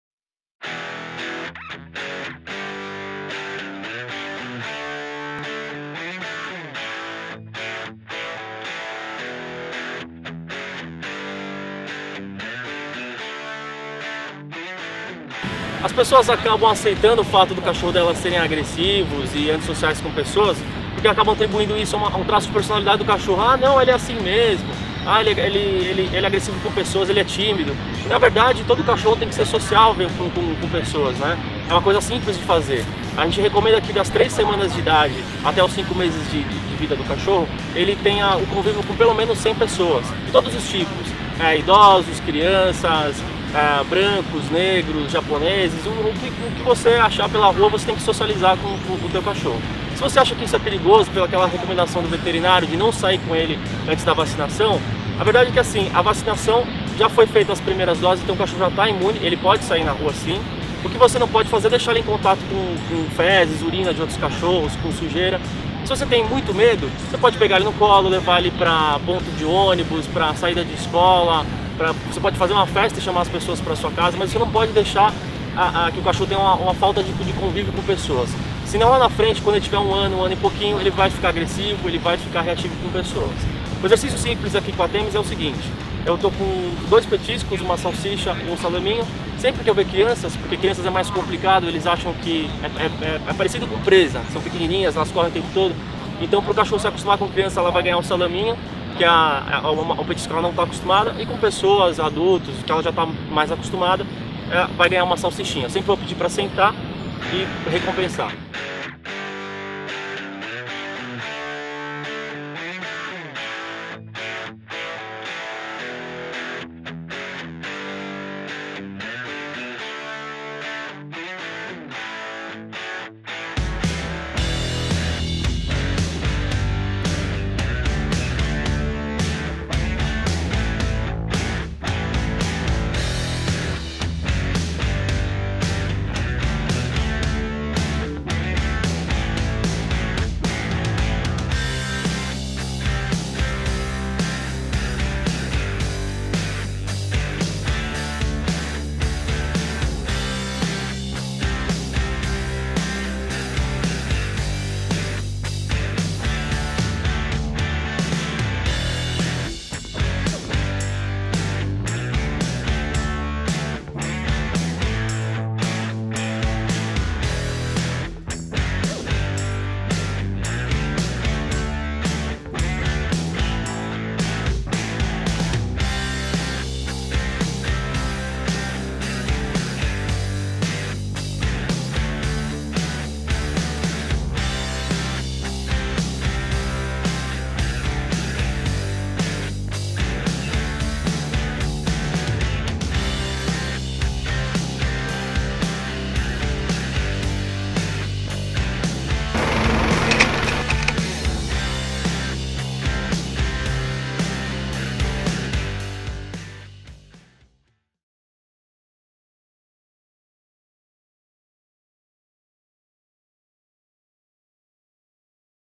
Socializando com Pessoas
Uma técnica simples e eficaz para socializar seu filhote com pessoas de todos os tipos e evitar que seu cachorro se transforme em um adulto reativo, medroso ou agressivo com pessoas.